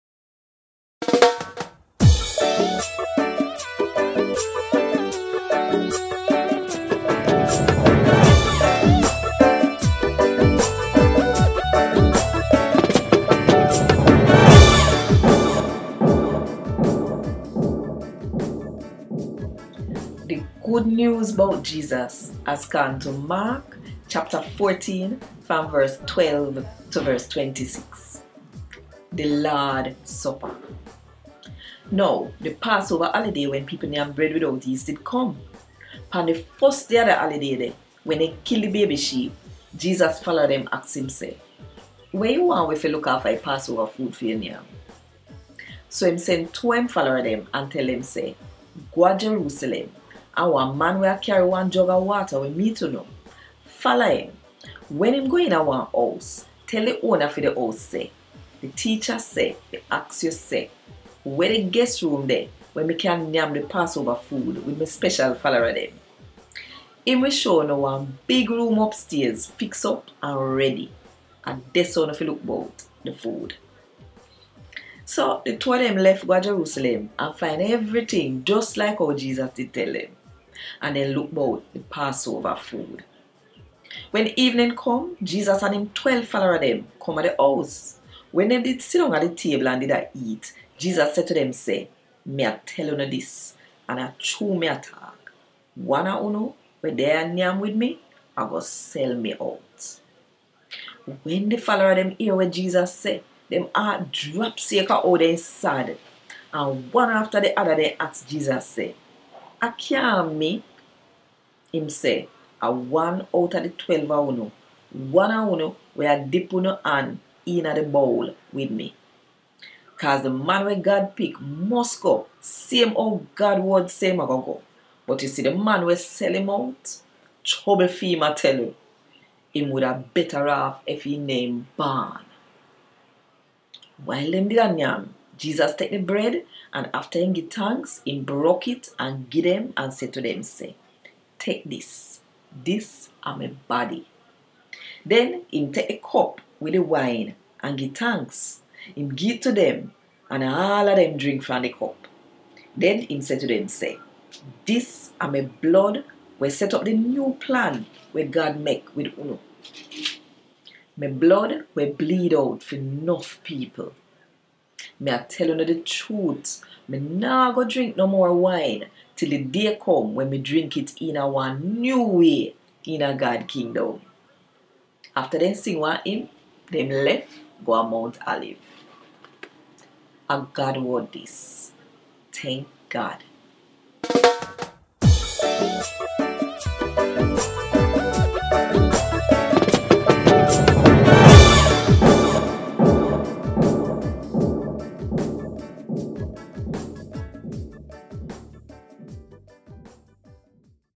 A Gad Wod Dis! Maak 14:12-26 (JNT)
Jo-Ann Richards reads from the Jamaican New Testament (aka the Patwa Bible) published by The Bible Society of the West Indies.

To learn more about CREW 40:4 our ministry focusing on culturally relevant expressions of worship, visit: